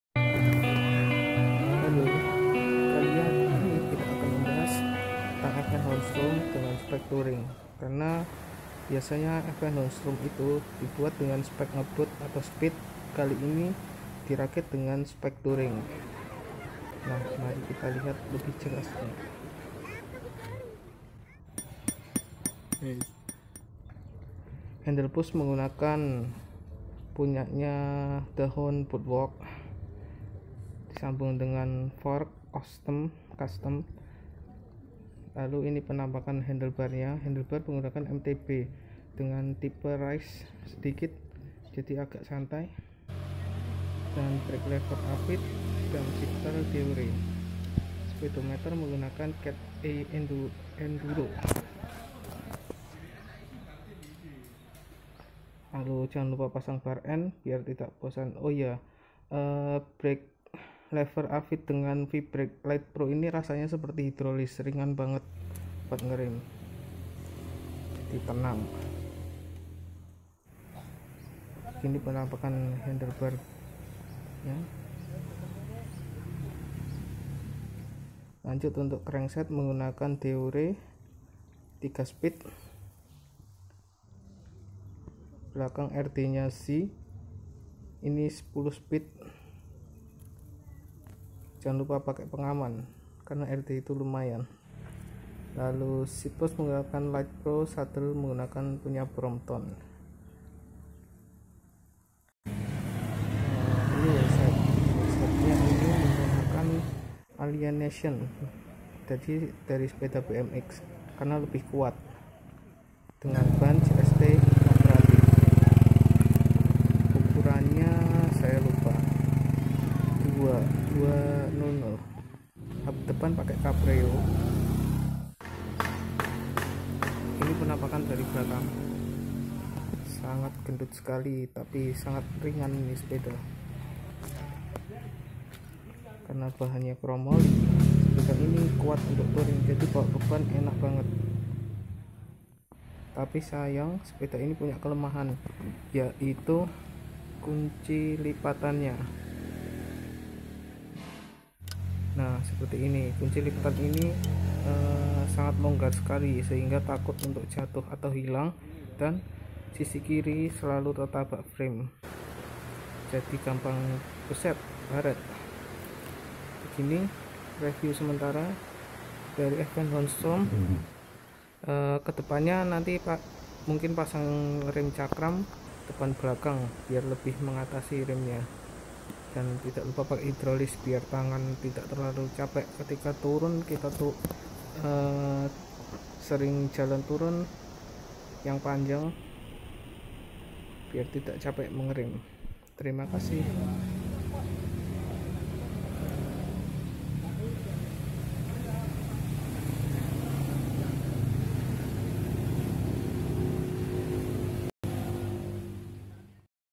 FnHon Storm Touring Setup
Kali ini FnHon Storm storm yang agak beda dengan kebanyakan yang ada. Bukan dirakit menjadi speed tapi dijadikan untuk touring? Rasanya?

Simak lebih mendalam dalam video ini